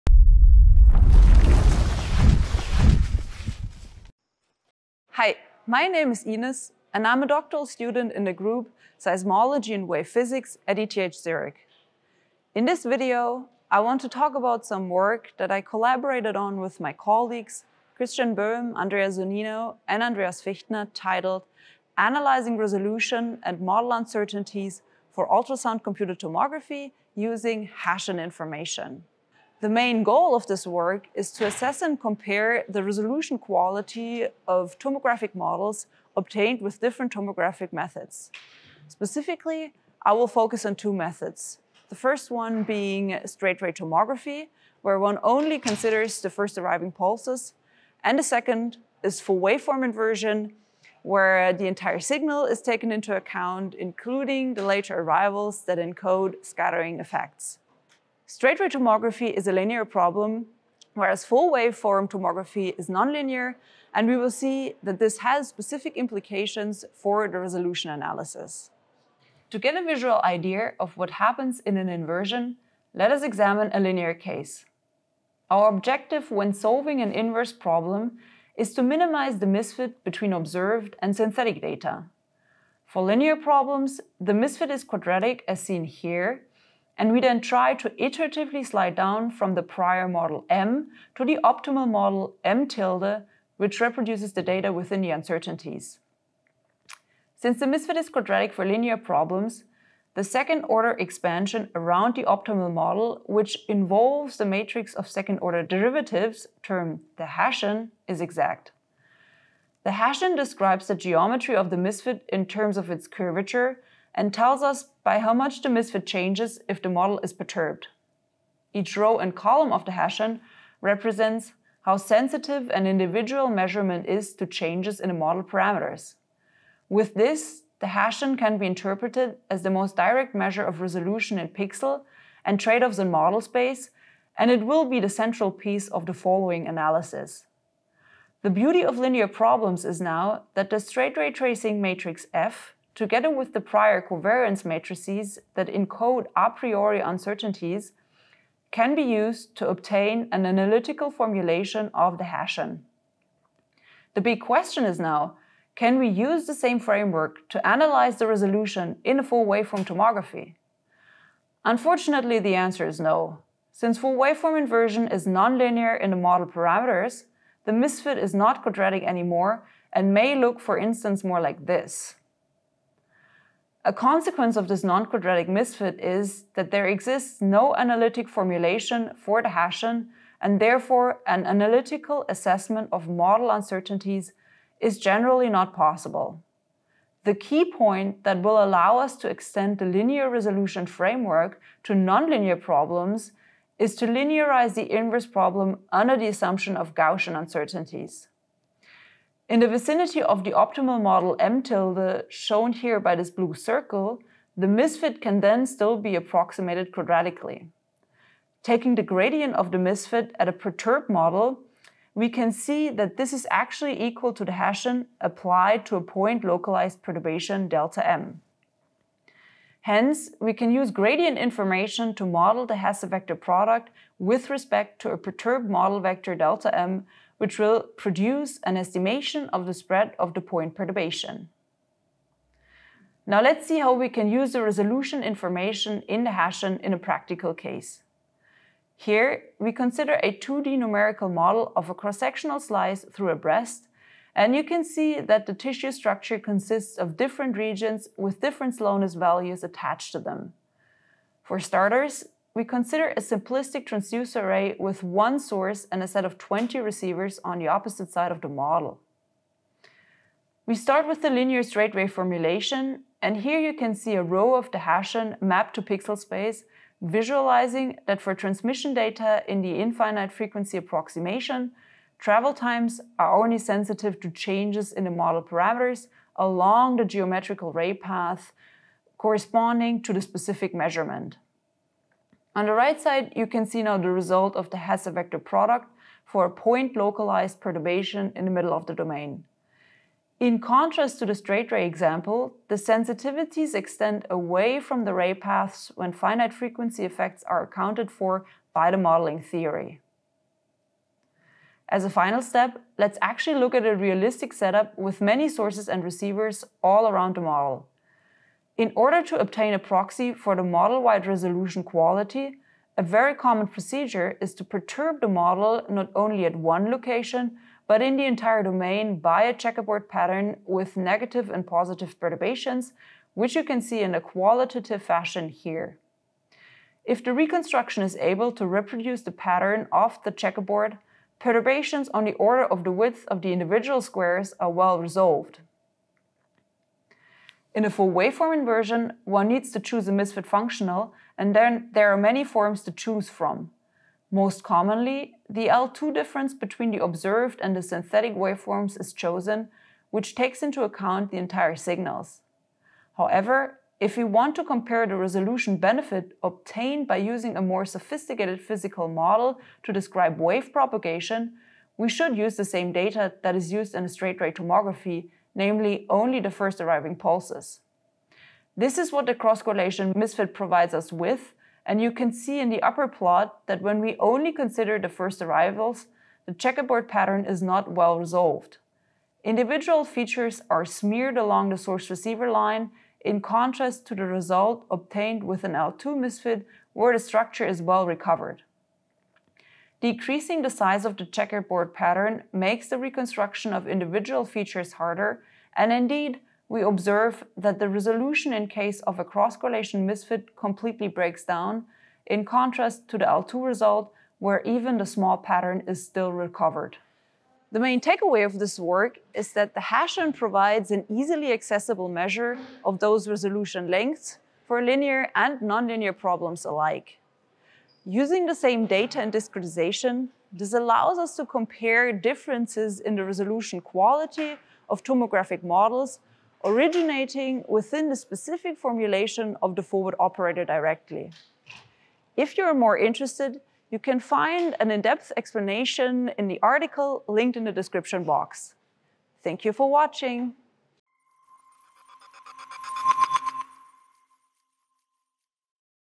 Estimating resolution lengths in tomographic models
Characterizing the spatial resolution and uncertainties related to a tomographic reconstruction are crucial to assess its quality and to delineate important features. For linear inverse problems such as straight-ray tomography, Bayesian inference provides a robust framework for resolution analysis in the spatial domain. This video explains how the framework can be generalized to non-linear inverse problems such as full-waveform inversion by exploiting gradient information to obtain a proxy for the resolution length in the reconstructed model.